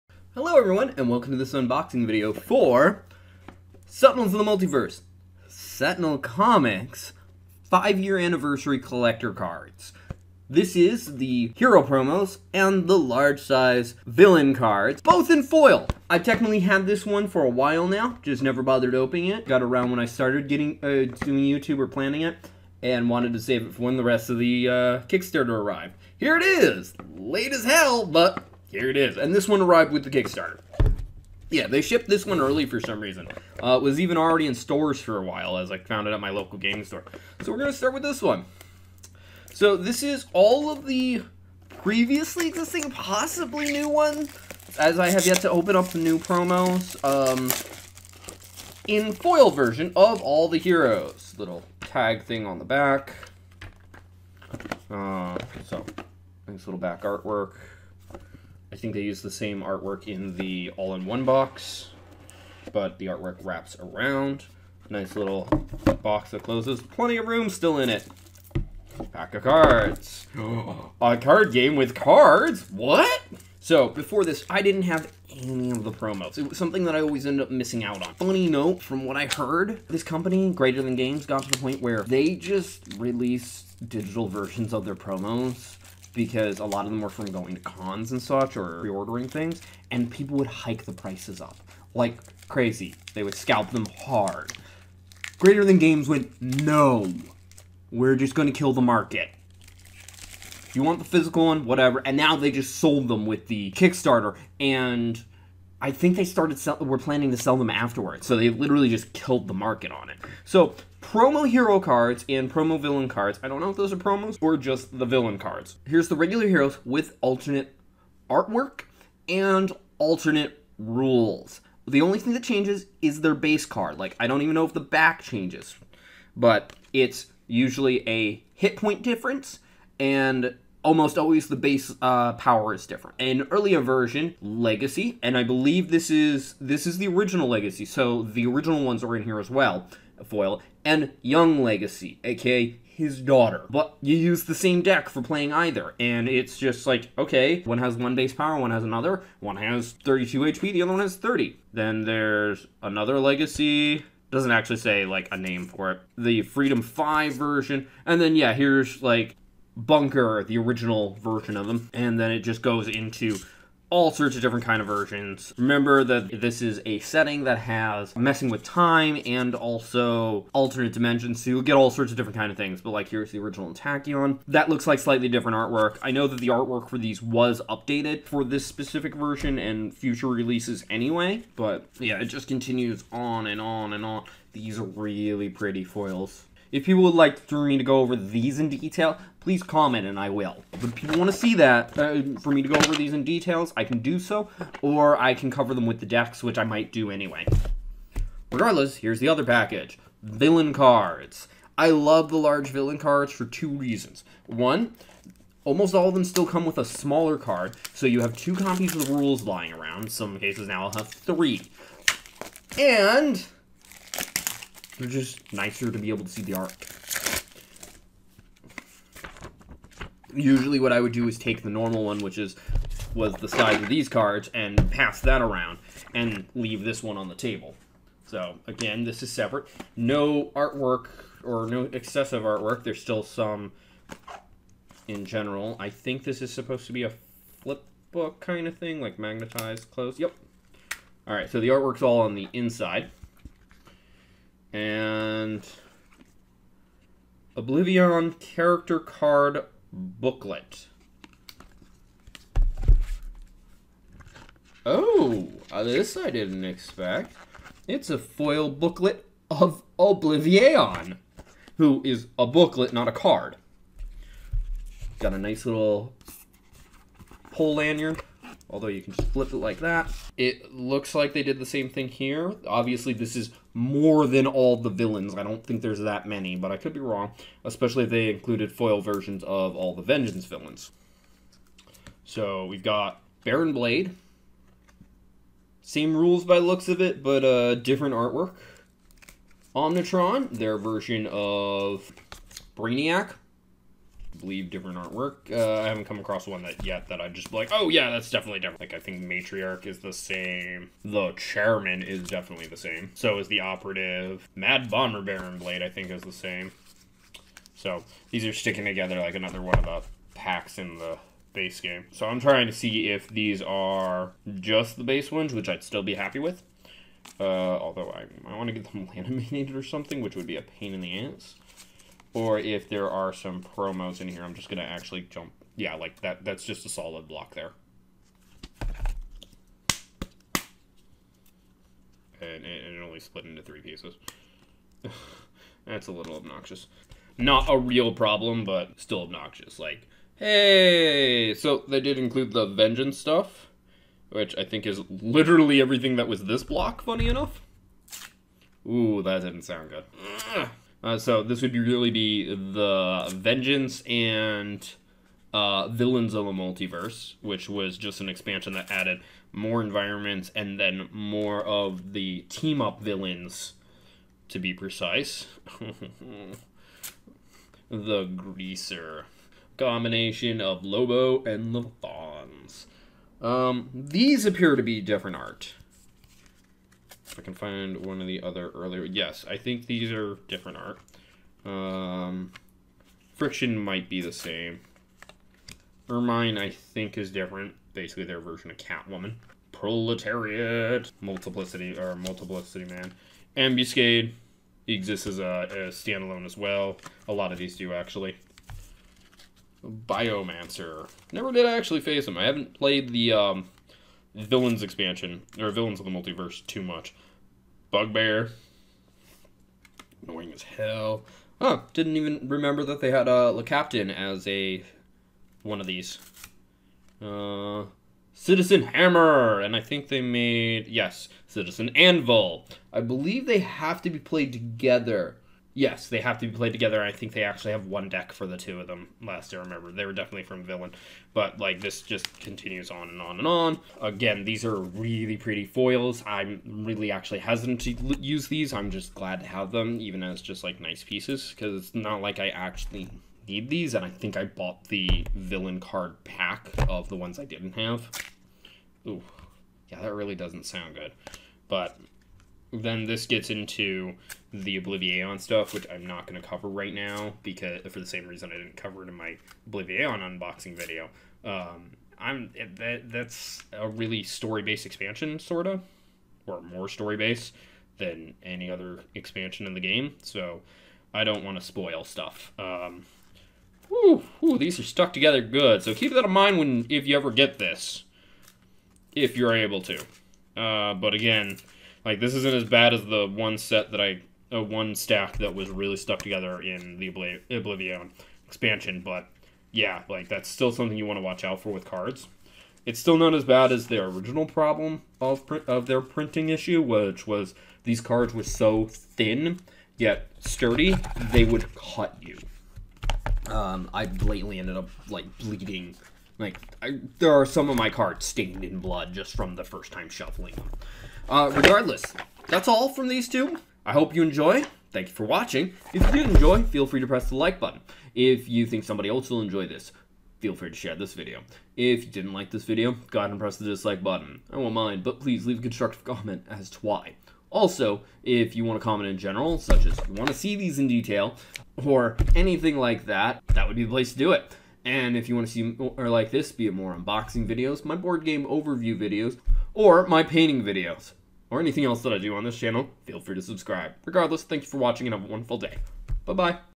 Unboxing Sentinels of the Multiverse Five Year Anniversary Foil Packs
Here is a video for the unboxing of the Sentinels of the Multiverse Five Year Anniversary Foil Packs, by Greater Than Games.  This covers both the Hero's and Villain's packs  This was a kickstarter order.